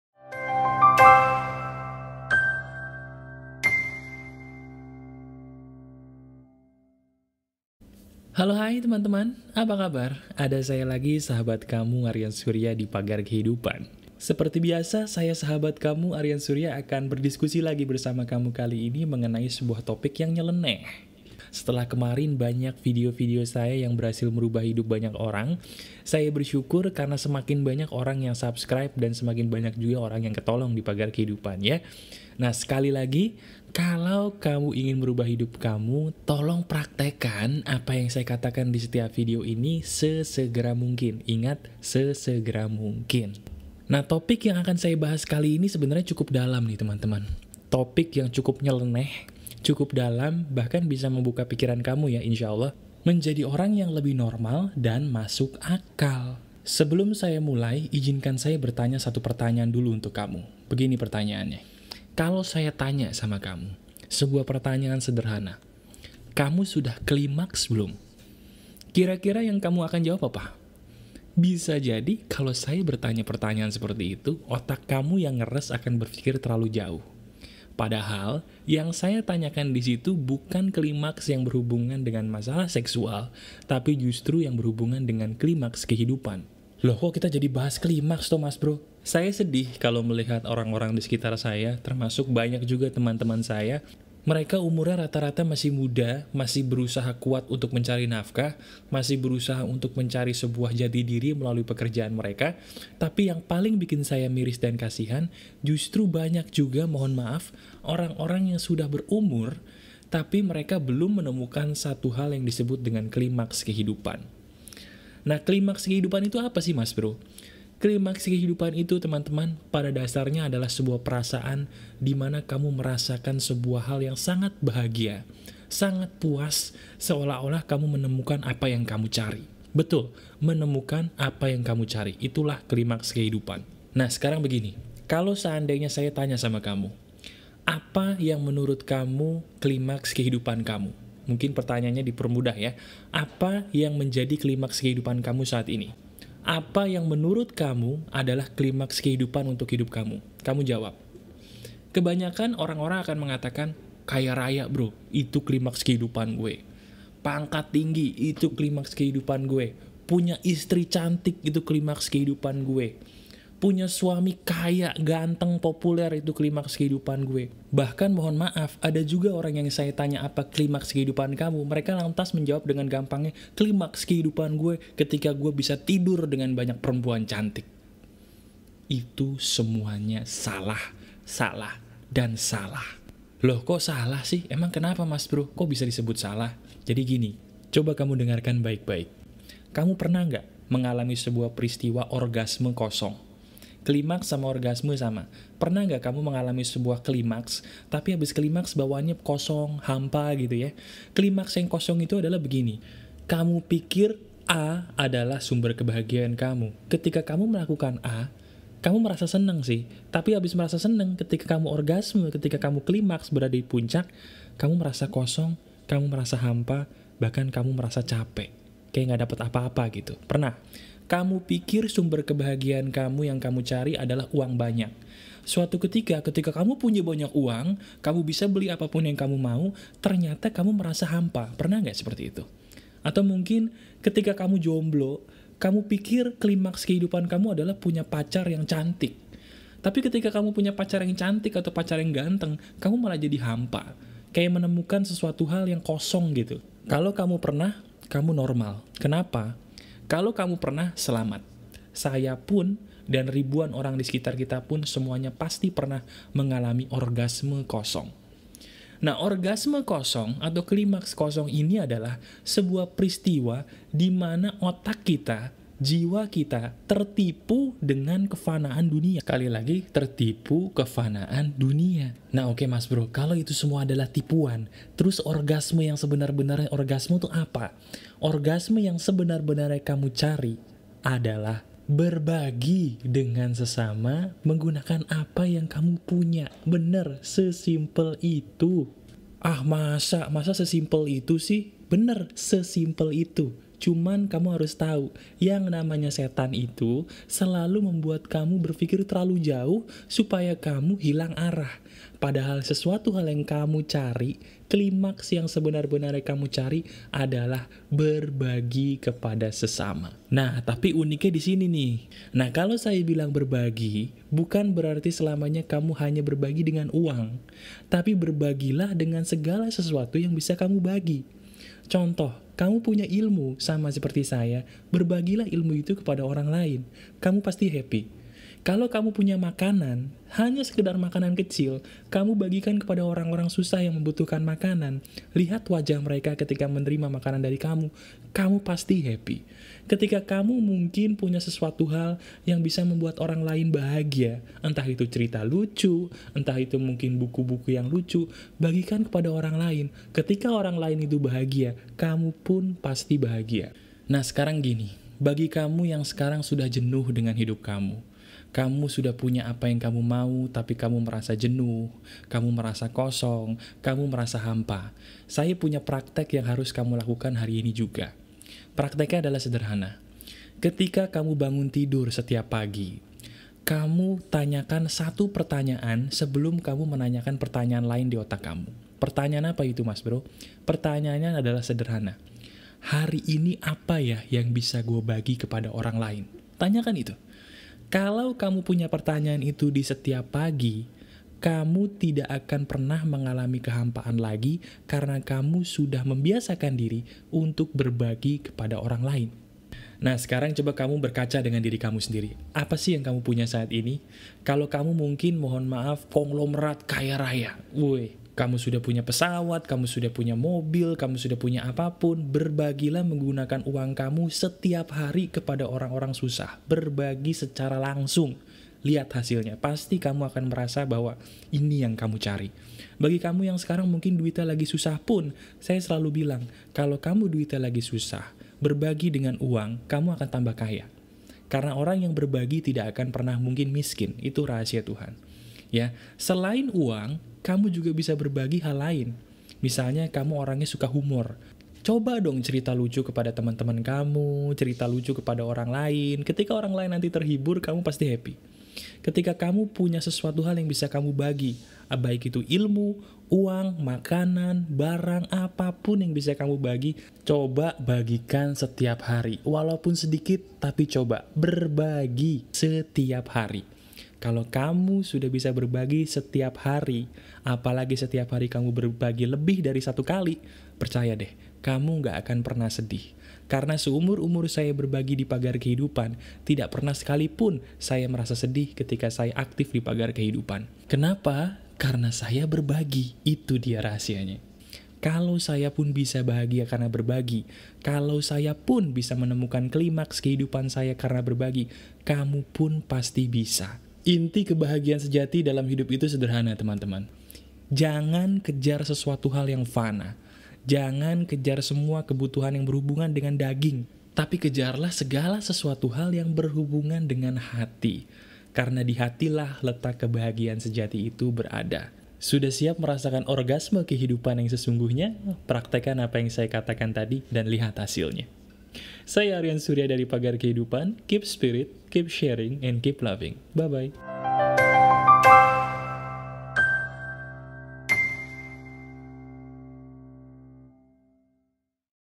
Motivasi Hidup Sukses - Sudah "Klimaks" Belum?
Sebuah motivasi hidup sukses dari Arian Surya pemilik Pagar Kehidupan. Di dalam video ini Arian Surya memberikan diskusi mengenai sebuah hal yang berhubungan dengan KLIMAKS! Nah, KLIMAKS apa yang dimaksud? Penasaran? Dengarkan ini! :)